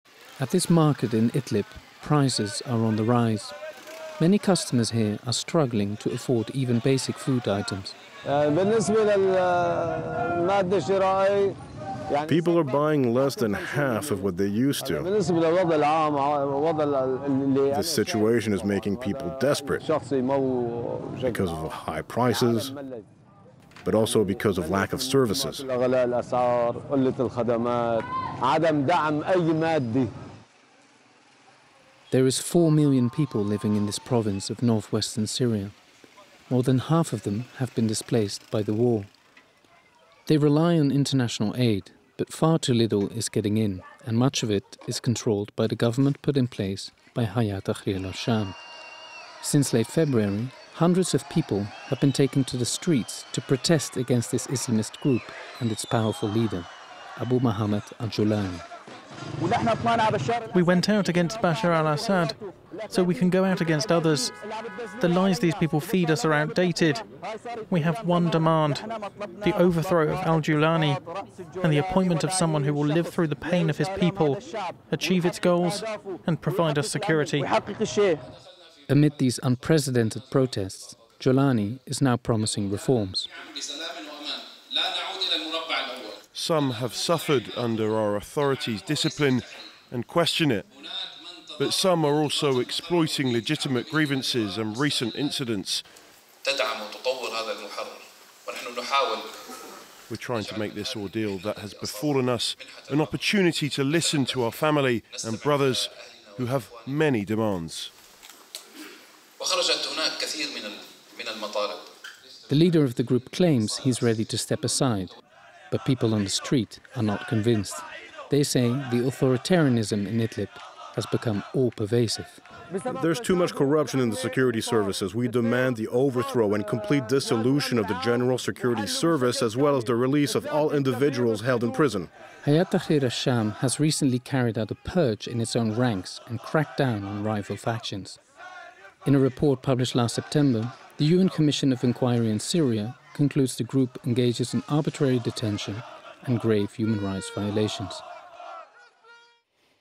Syria: Protesters march against Islamist group Hayat Tahrir al-Sham in Idlib • FRANCE 24 English
Protests grow in the northwest of Syria against the Islamist group Hay-at Tahrir Al-Sham. The group that controls the last opposition stronghold against Damascus is facing accusations of economic mismanagement and authoritarianism.